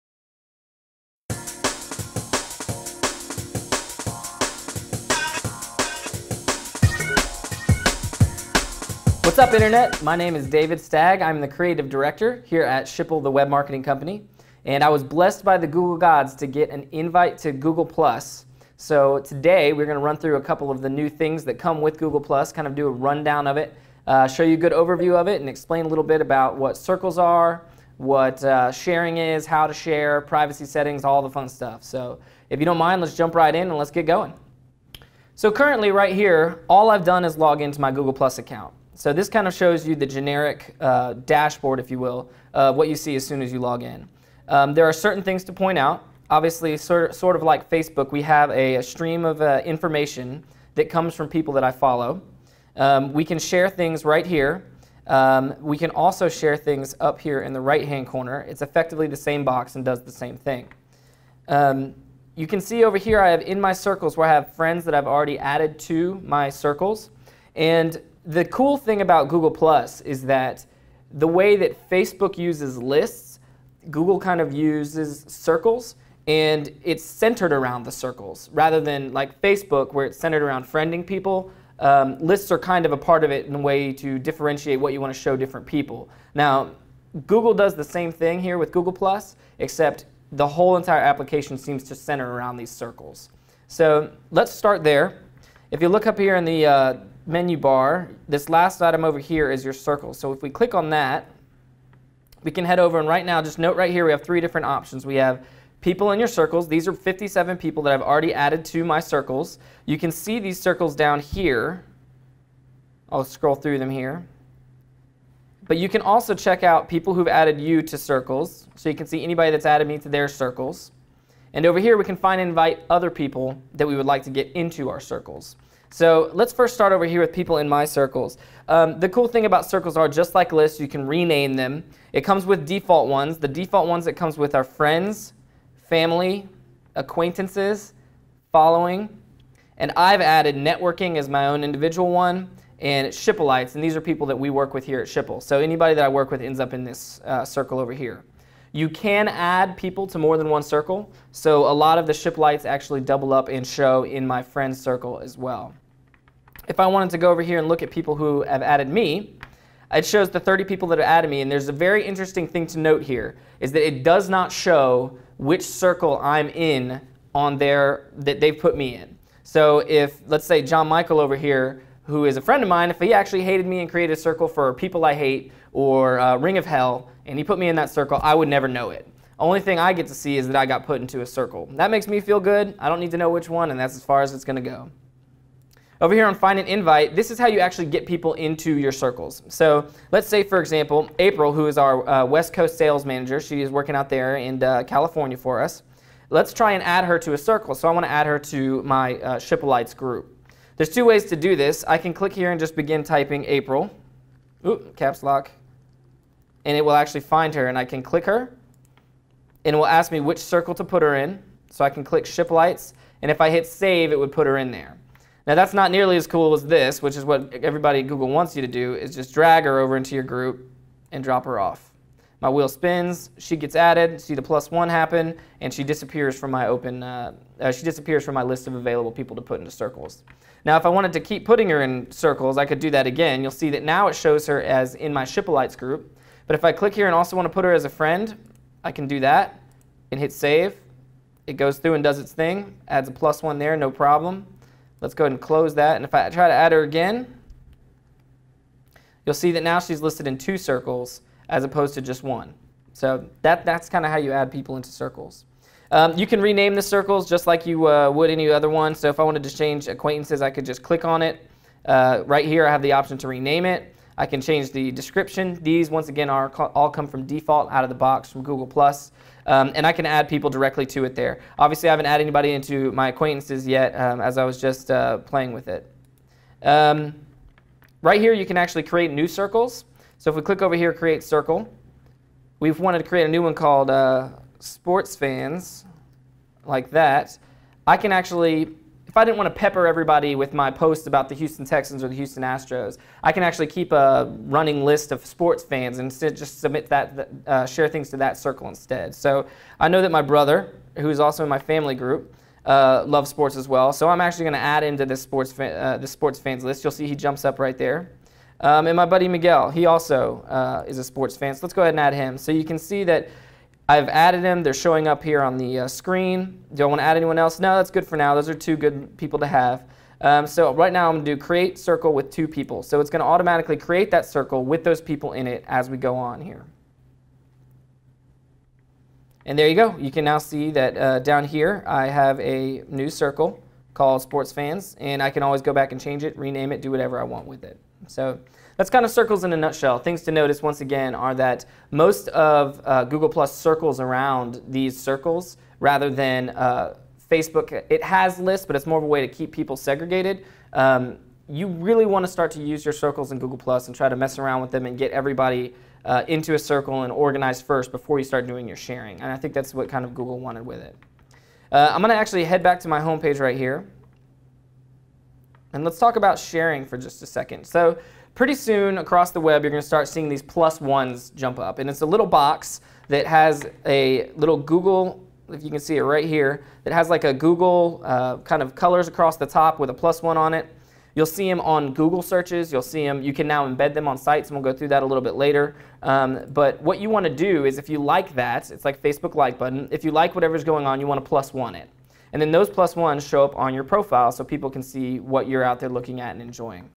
Intro to Google Plus with David Stagg
Learn how to use Google+, the latest social network to launch. Schipulite, David Stagg, discusses the differences between Google+ and Facebook. Find out what Circles are and how to use them. Learn what the Google +1 button is all about and why it's important to add to your website or blog. For more information on the Google +1 button, view our "Google +1 Button Overview" video